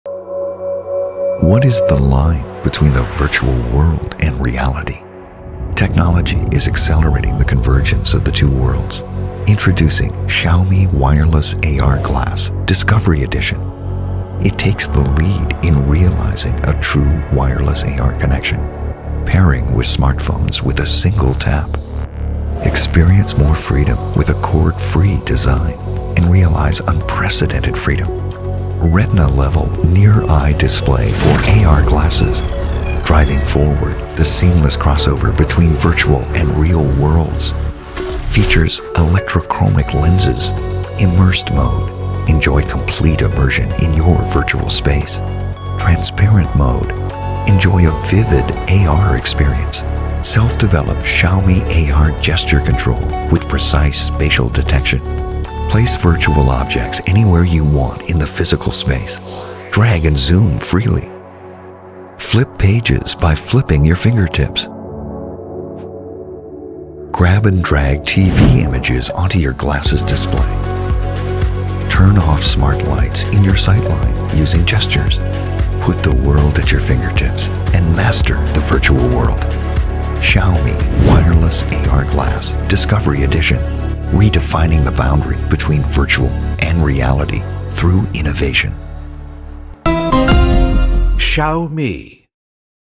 Xiaomi Wireless AR Glass Discovery Edition
Virtuelle Welt und Realität verschwimmen mit der Xiaomi Wireless AR Glass Discovery Edition!
Platziere virtuelle Objekte überall in deinem physischen Raum und tauche in eine neue Welt ein – fortschrittliche Technologie für ein nahtloses digitales Erlebnis! 😎